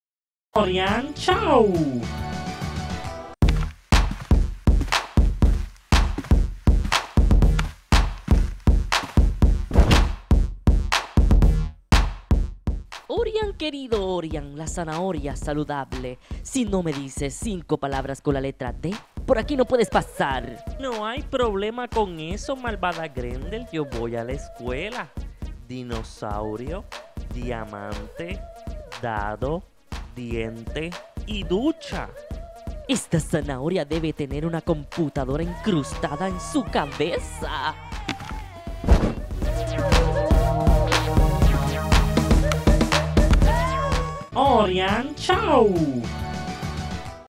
5 palabras con la letra D...educativo infantil
Esta es una produccion de Luis villafane Pinto
hecho en Puerto Rico
transmitido por Tiva Tv canal 8